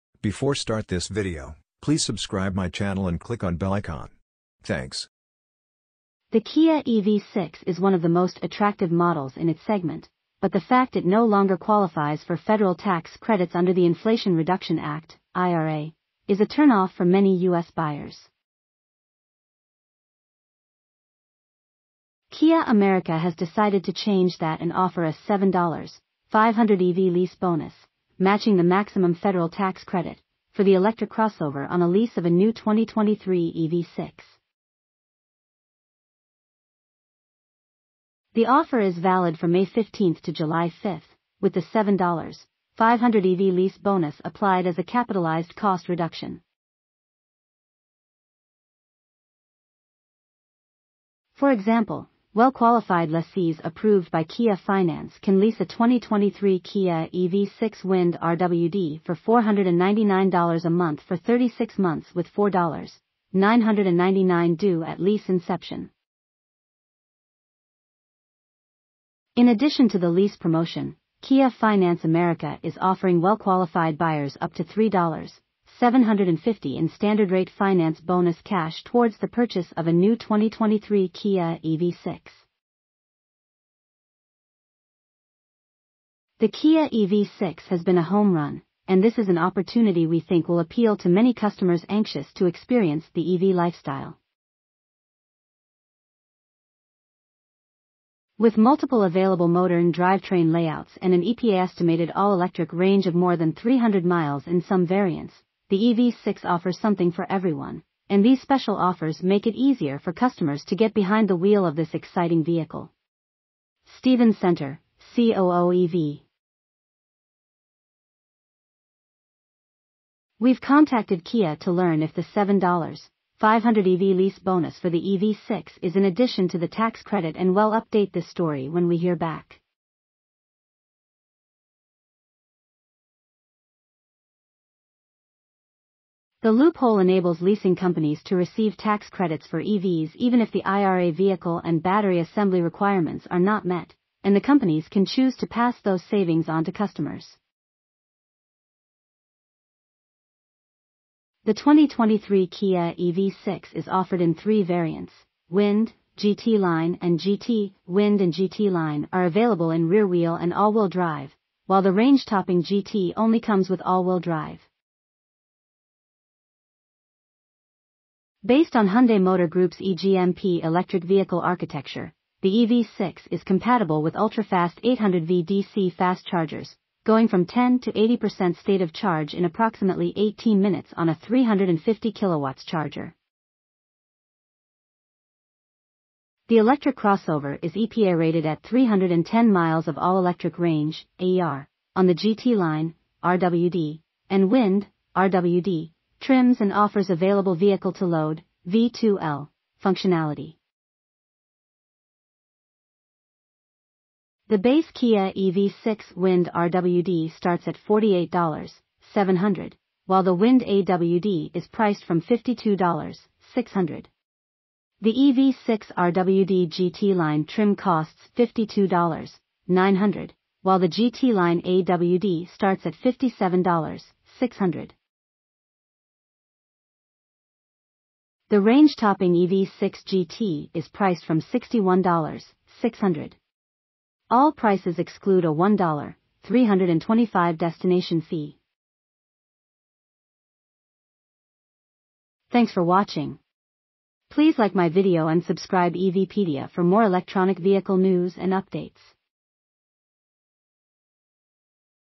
Kia Makes 2023 EV6 More Attractive To Lease And Purchase In US
The Kia EV6 is one of the most attractive models in its segment, but the fact it no longer qualifies for federal tax credits under the Inflation Reduction Act (IRA) is a turn off for many US buyers. Kia America has decided to change that and offer a $7,500 EV lease bonus – matching the maximum federal tax credit – for the electric crossover on a lease of a new 2023 EV6. The offer is valid from May 15 to July 5, with the $7,500 EV lease bonus applied as a capitalized cost reduction. For example, well-qualified lessees approved by...